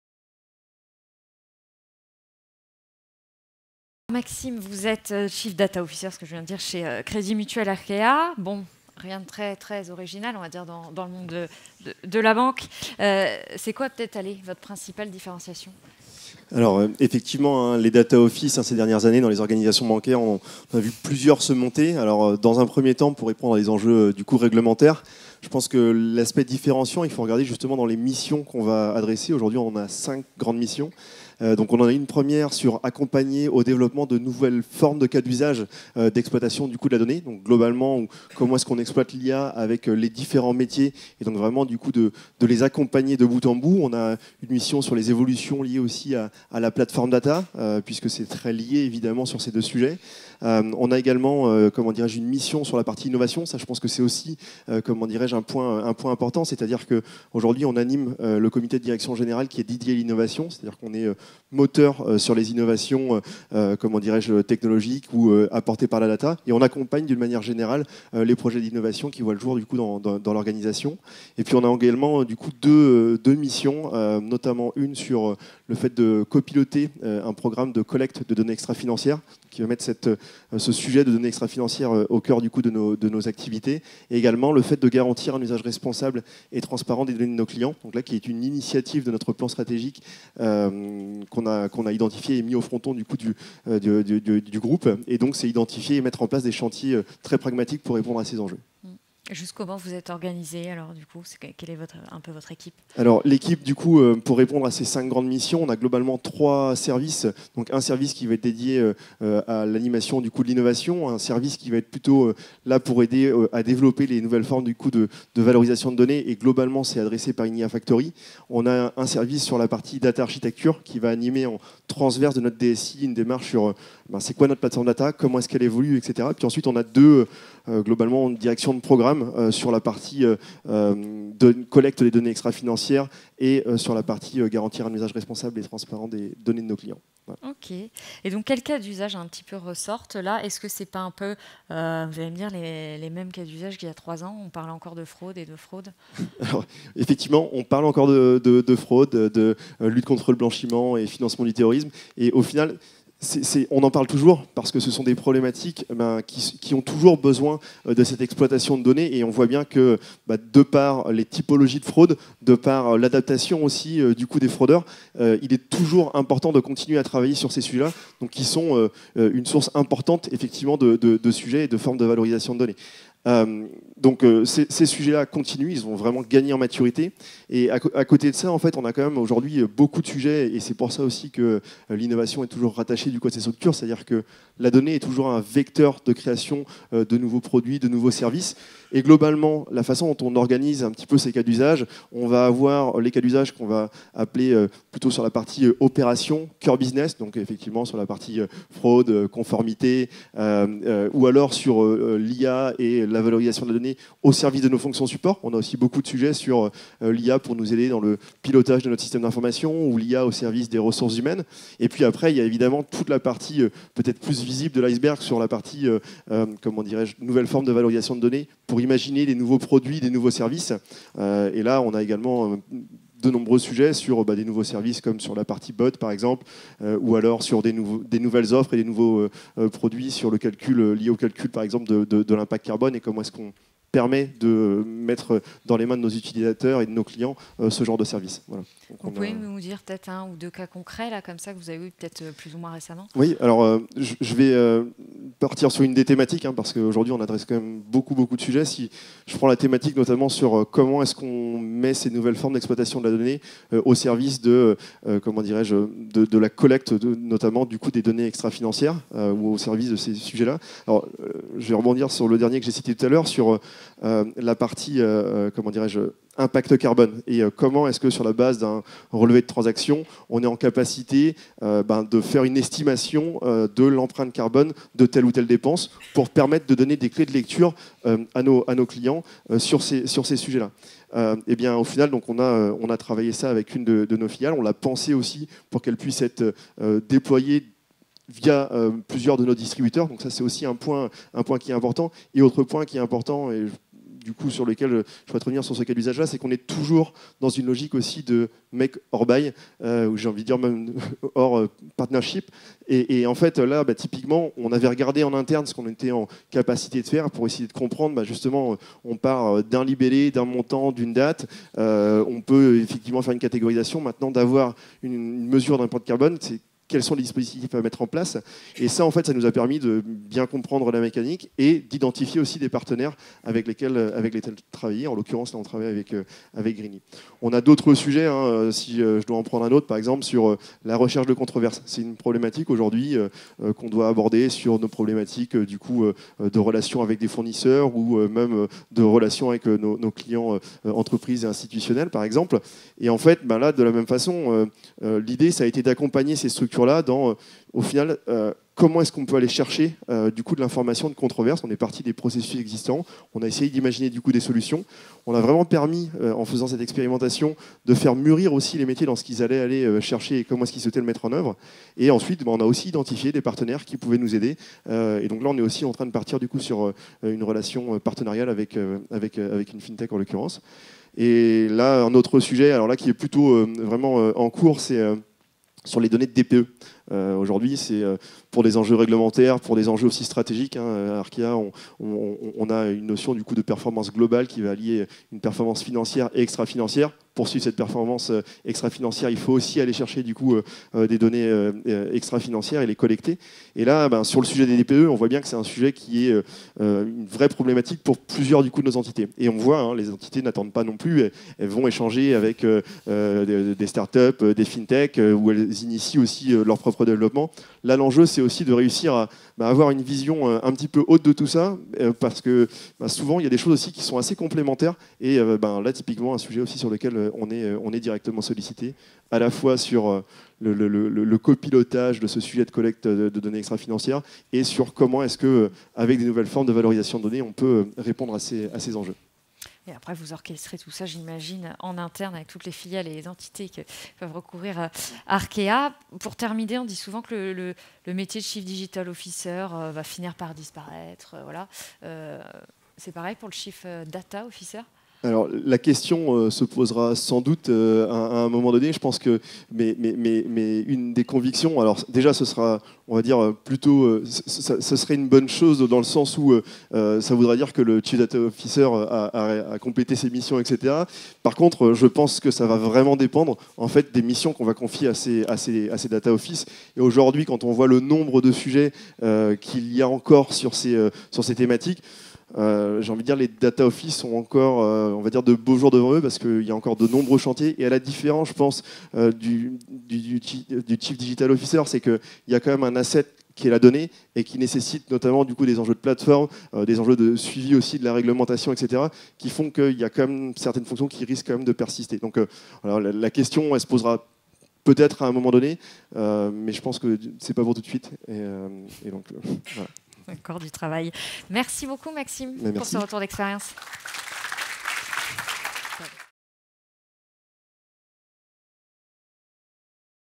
FFT22 | L’open innovation au sein d’un data office | FinTech R:Evolution CROSSROADS
Plénière Tech et métiers | Interview
Data et UX : le modèle partenarial pour expérimenter de nouveaux usages bancaires.

Intervenant : Maxime Havez, Chief Data Officer, Crédit Mutuel Arkéa

Interviewé par Charlie Perreau, responsable du service start-up, Les Echos